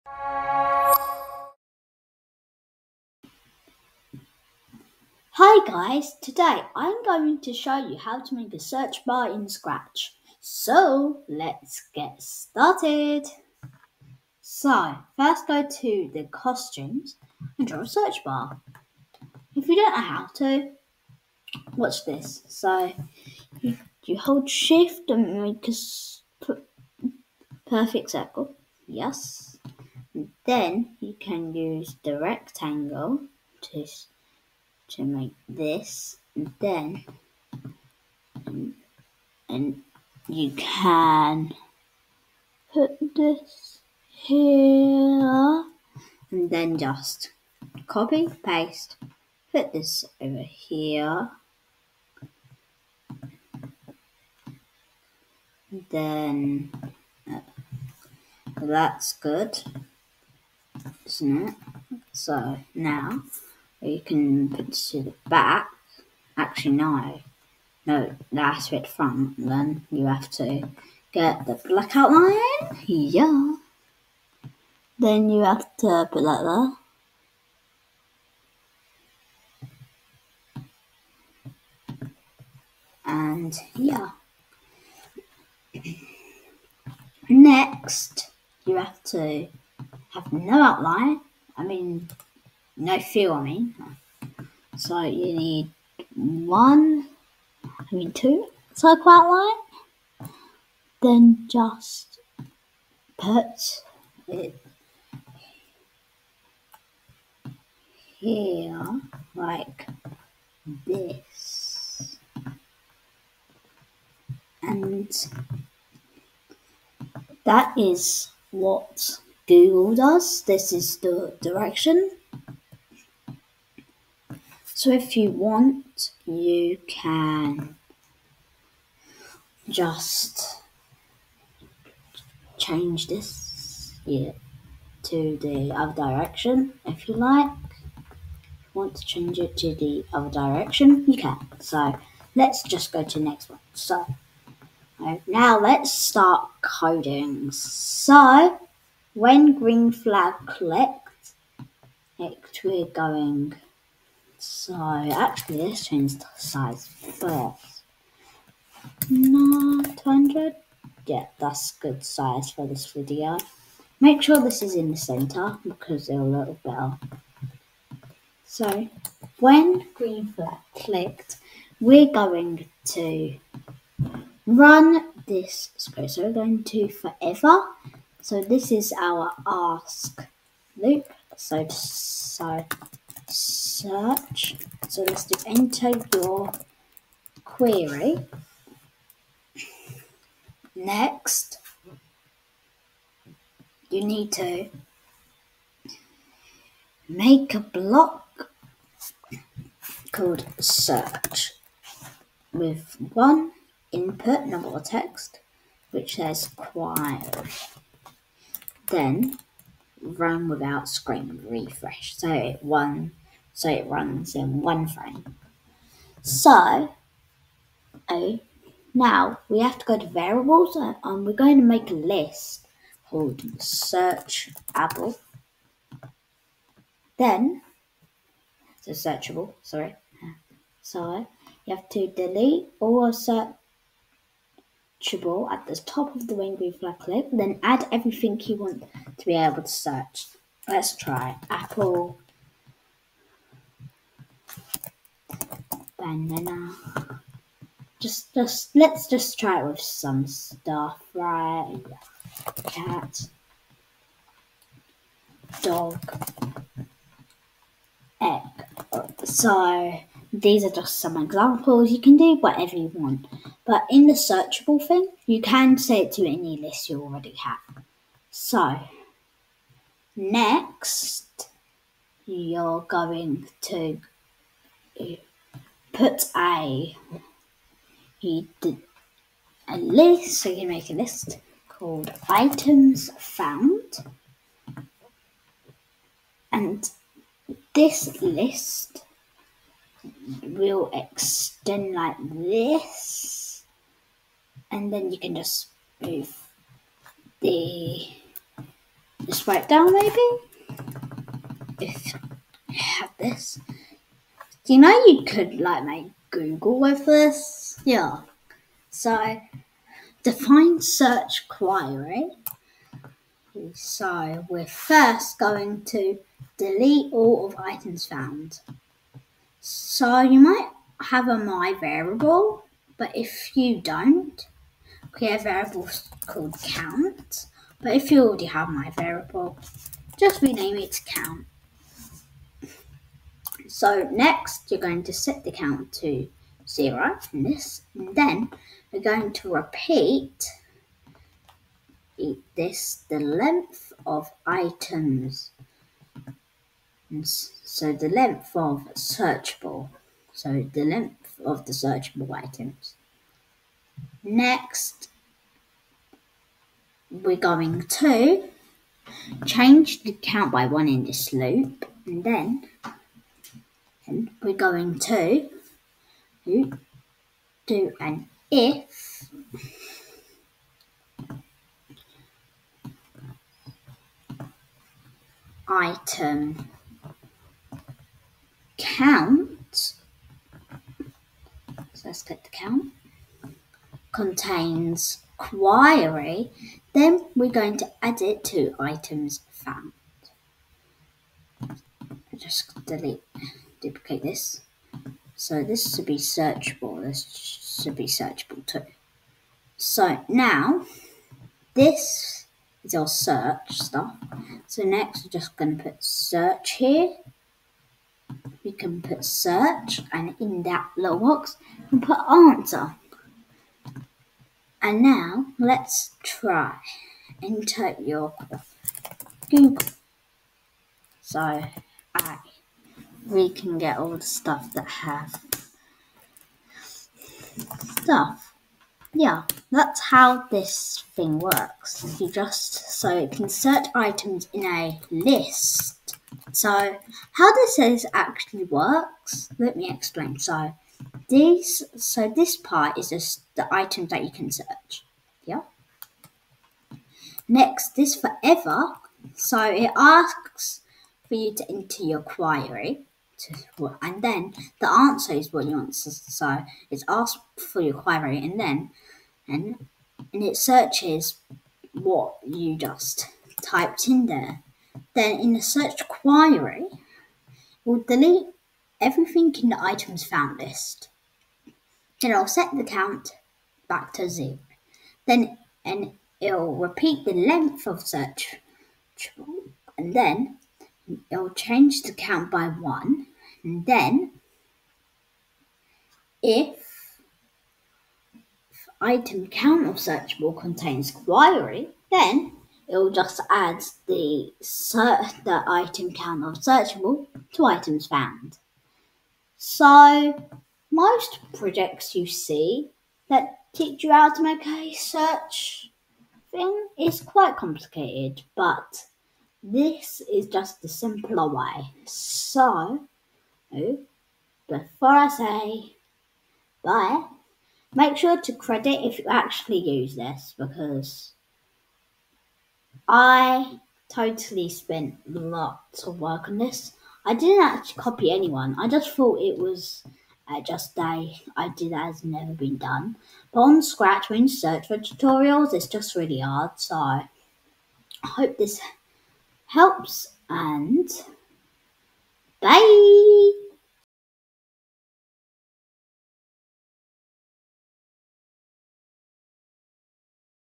You Won't Believe How Easy It Is to Make a Search Bar in Scratch - Tutorial
Learn how to make a search bar in Scratch that works just like Google! This tutorial will show you how easy it is to create a search feature in your Scratch projects.